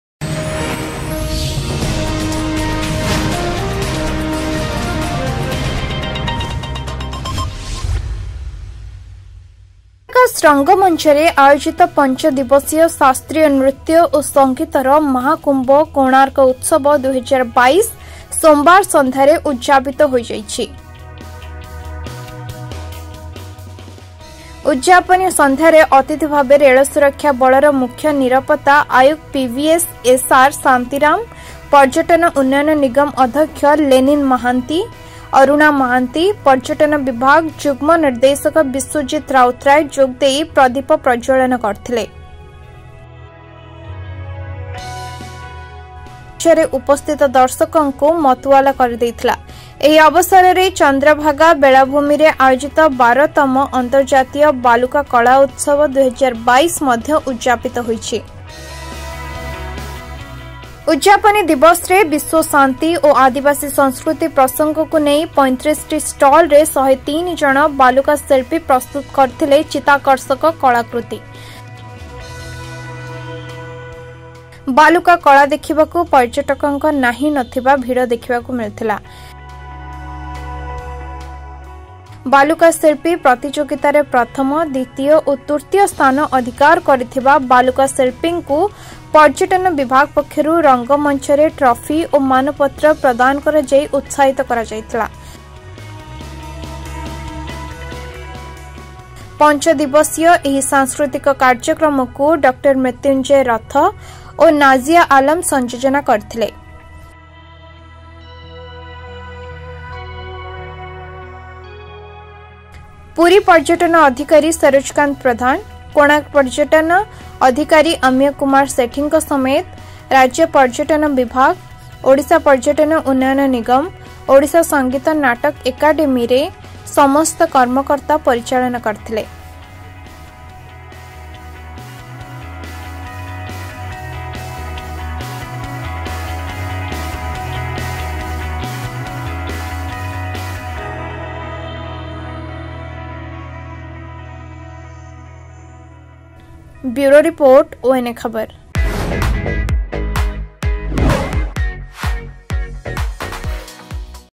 Konark: Konark Festival & International Sand Art Festival-2022
Konark: Konark Festival and International Sand Art Festival-2022 was inaugurated
About Channel:
ଚ୍ୟାନେଲ୍ ବିଷୟରେ.